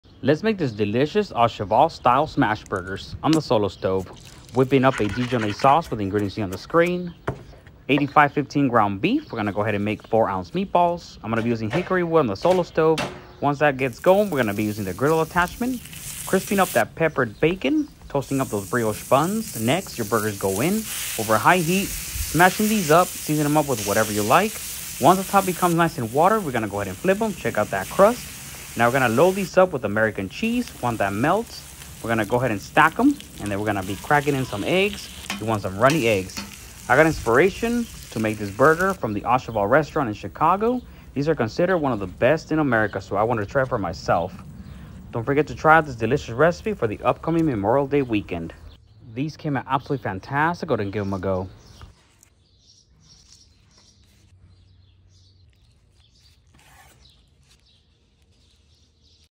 Au Cheval style burgers on the
Au Cheval inspired burgers were absolutely amazing on the @solostove
Want to try an amazing for this upcoming Memorial Day? Then look no further than to make this amazing burger. Loaded with thick sliced peppered bacon, a runny egg and dijonnaise sauce.  I was inspired as this burger is considered one of the best in the US.

Use my link to shop or click the link in my stories or bio